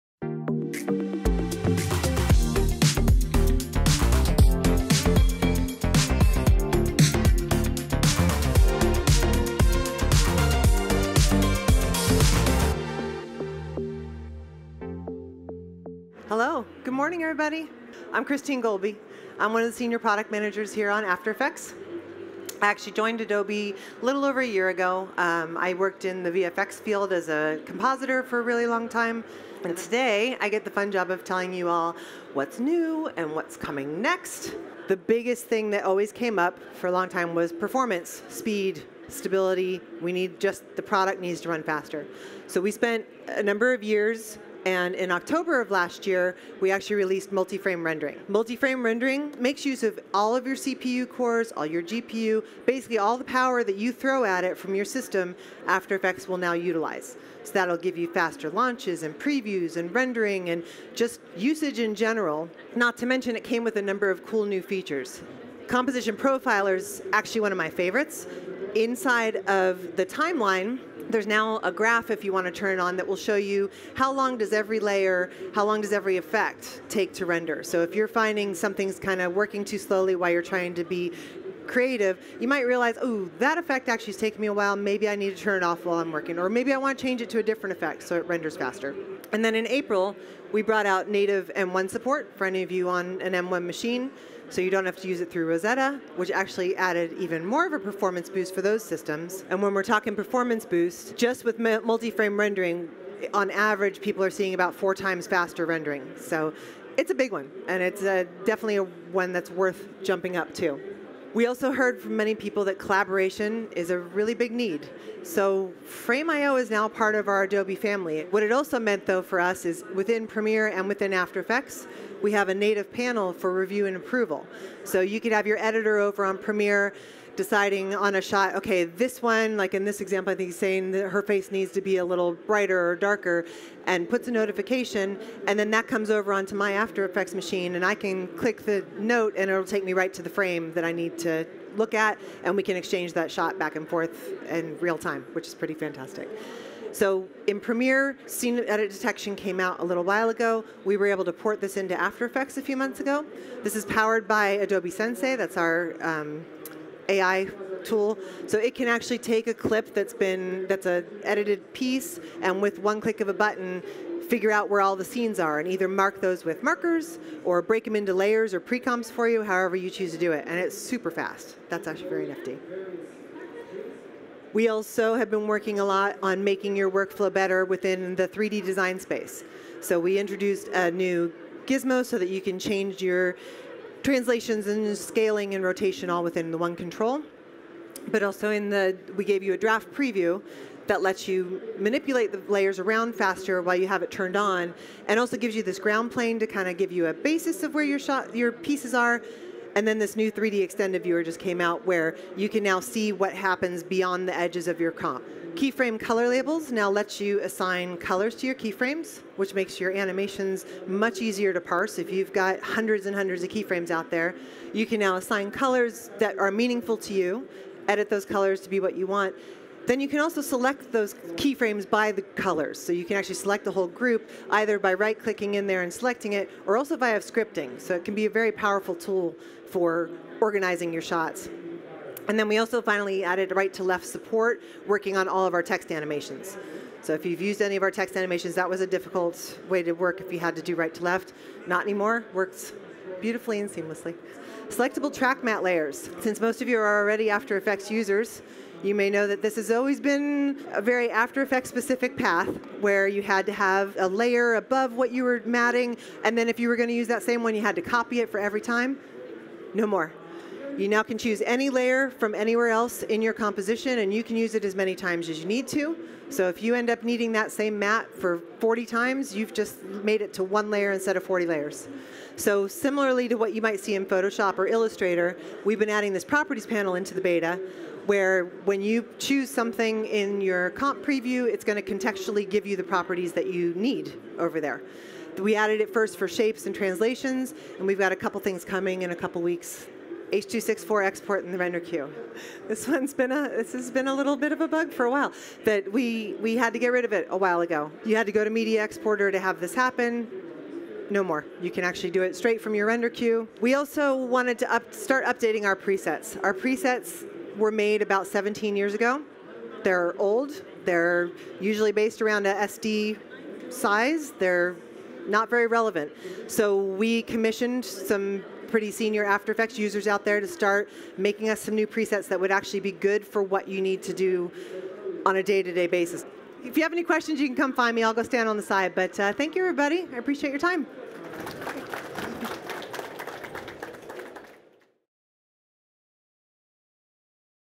After Effects: What's New in Motion Design | IBC 2022 | Adobe Video
Now up to three times faster on Apple M1 Macs, After Effects gives today’s motion designers the tools they need to do great work — and the speed to finish sooner. Along with support for Apple silicon, the latest release of After Effects offers built in review and approval tools with Frame.io for Creative Cloud, Scene Edit Detection, powered by Adobe Sensei and improvements for 3D workflows.